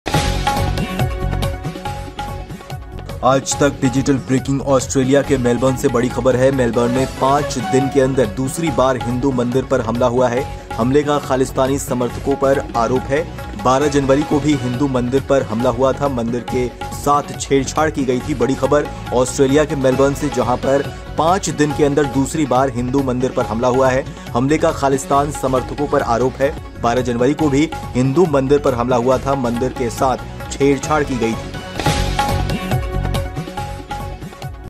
Breaking News: मेलबर्न में हिंदू मंदिर पर दूसरी बार हमला, खालिस्तान समर्थकों ने की तोड़फोड़ !
ऑस्ट्रेलिया में हिंदू मंदिरों पर हमले रुकने का नाम नहीं ले रहे हैं. 12 जनवरी को मेलबर्न में एक हिंदू मंदिर पर हमला हुआ था, इसके ठीक 5 दिन बाद मेलबर्न में ही दूसरे हिंदू मंदिर को शिकार बनाया गया. वारदात को अंजाम देने के आरोप खालिस्तान समर्थकों पर लग रहे हैं.

Attacks on Hindu temples in Australia are not taking the name of stopping. On January 12, a Hindu temple was attacked in Melbourne, just 5 days later another Hindu temple was made a victim in Melbourne itself. Khalistan supporters are being accused of carrying out the incident.